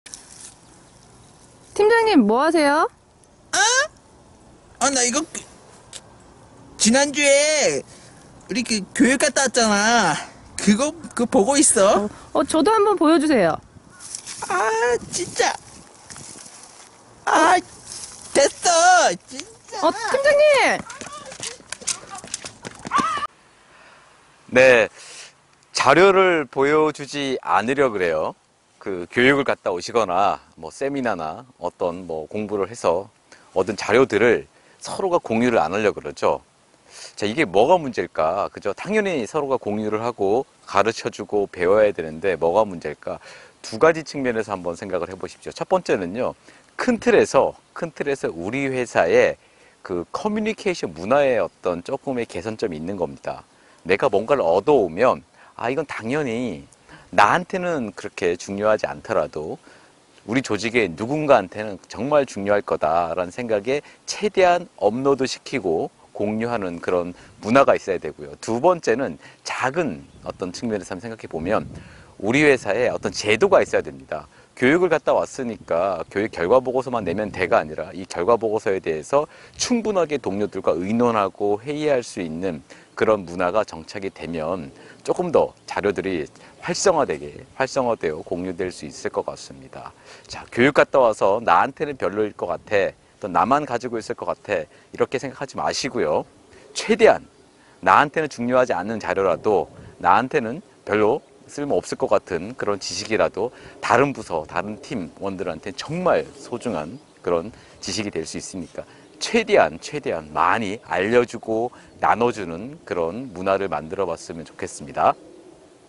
정보공유 좀 합시다[김컨]
교육 또는 세미나를 갔다와서 또는 공부를 해서 얻은 정보들을
공유를 잘 안하려고 하죠.  나에게만 필요할 자료일 것 같아서 또는  유용한 자료가 아닐 것 같아서... 이런 자료들이 다른  팀, 다른 부서에서는 필요한 자료일 수 있습니다. 우리 회사의 활발한 정보공유를 위해서  커뮤케이션 문화나 제도를 개선해보세요.
이 영상이 마음에 드셨다면 '구독','좋아요' 눌러주세요^^